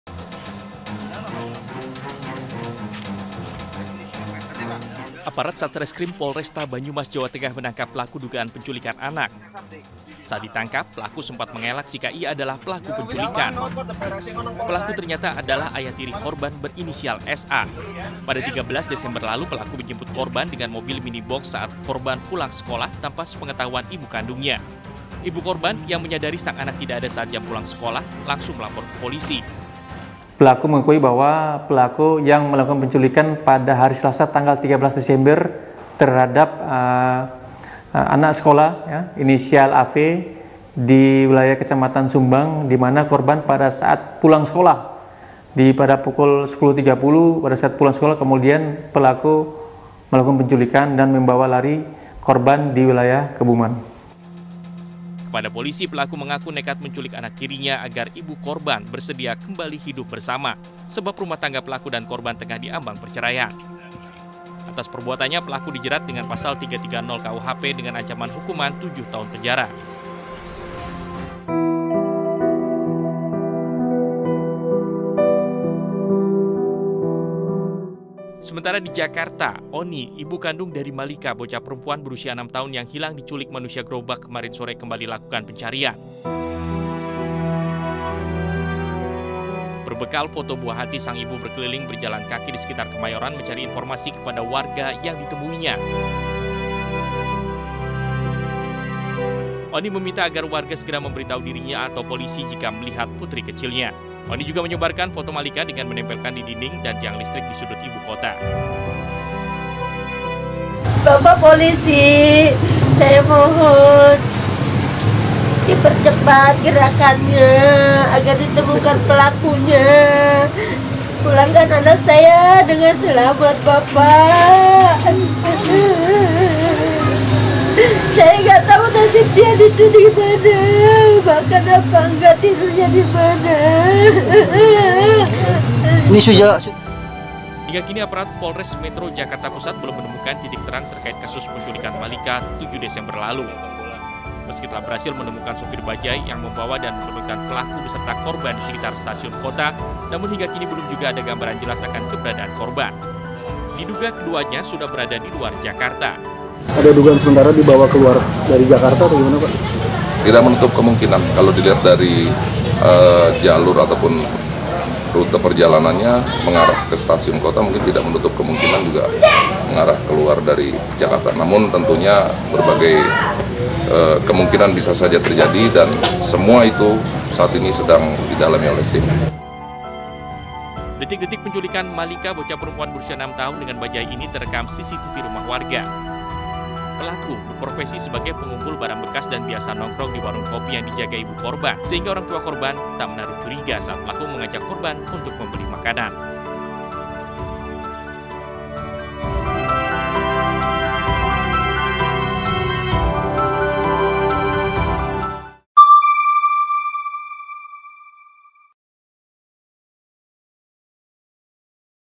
Penculikan Anak Kian Marak, Pelaku Bisa dari Orang Dekat Korban
Seorang pelaku penculikan anak berhasil ditangkap polisi di Banyumas, Jawa Tengah. Ironisnya pelaku merupakan ayah tiri korban. Sementara di Jakarta, keluarga Malika bocah 6 tahun yang diculik manusia gerobak terus berusaha mencari keberadaan sang buah hati.

Android: inews.id/android
iPhone: inews.id/ios